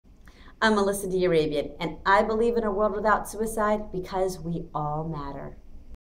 Melissa d'Arabian Believes in a World Without Suicide
Food Network star and New York Times best selling cookbook author Melissa d'Arabian supports AFSP's World Without Suicide campaign.  Melissa was also the recipient of AFSP's Lifesavers Survivor Award in 2013. To learn more about suicide prevention, please visit our